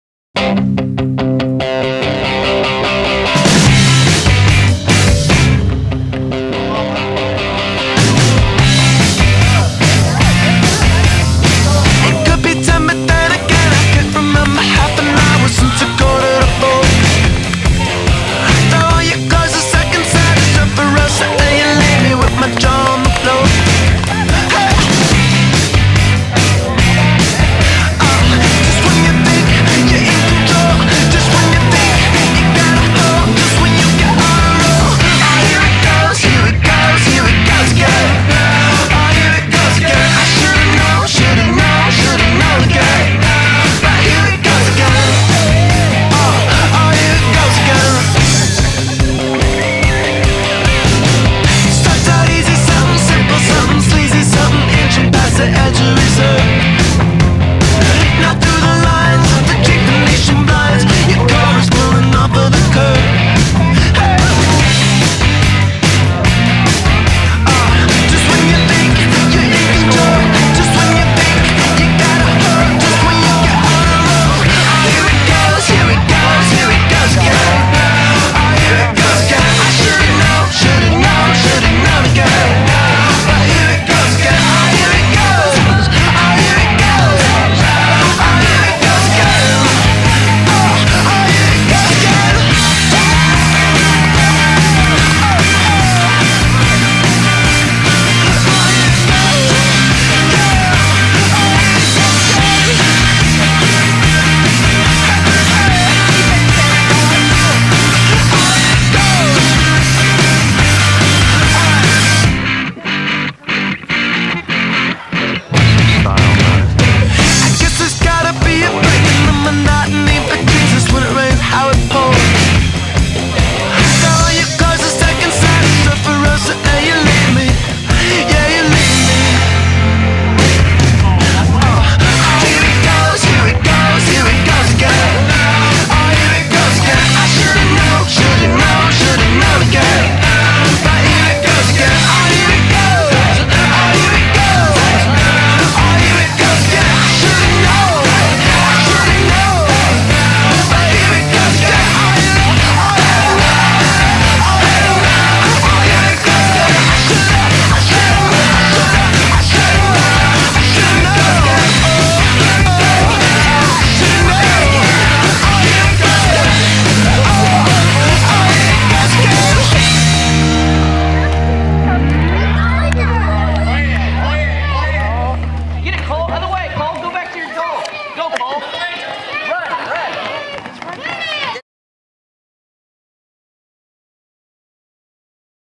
Soccermania -- Starring Aaron Wildoner
Video of Aaron playing soccer on 1/31/2009.